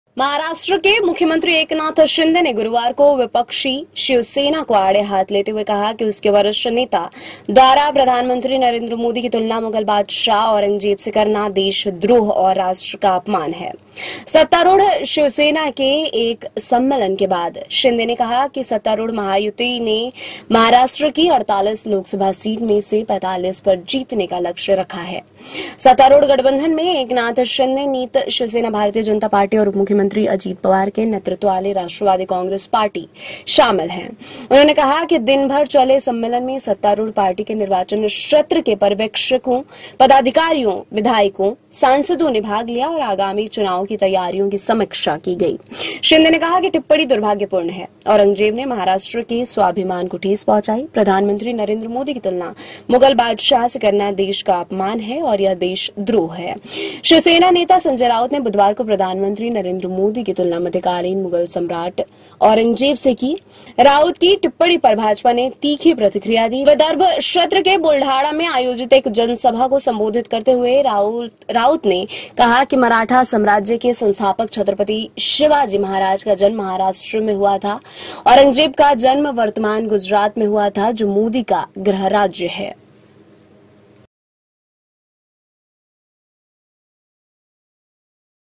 PM Modi की औरंगजेब से तुलना देश का अपमान: Eknath Shinde
महाराष्ट्र के मुख्यमंत्री एकनाथ शिंदे ने गुरुवार को विपक्षी शिवसेना को आड़े हाथ लेते हुए कहा कि उसके वरिष्ठ नेता द्वारा प्रधानमंत्री नरेंद्र मोदी की तुलना मुगल बादशाह औरंगजेब से करना देशद्रोह और राष्ट्र का अपमान है।

सत्तारूढ़ शिवसेना के एक सम्मेलन के बाद शिंदे ने कहा कि सत्तारूढ़ 'महायुति' ने महाराष्ट्र की 48 लोकसभा सीट में से 45 पर जीतने का लक्ष्य रखा है। सत्तारूढ़ गठबंधन में एकनाथ शिंदे नीत शिवसेना, भारतीय जनता पार्टी और उपमुख्यमंत्री अजीत पवार के नेतृत्व वाली राष्ट्रवादी कांग्रेस पार्टी शामिल हैं।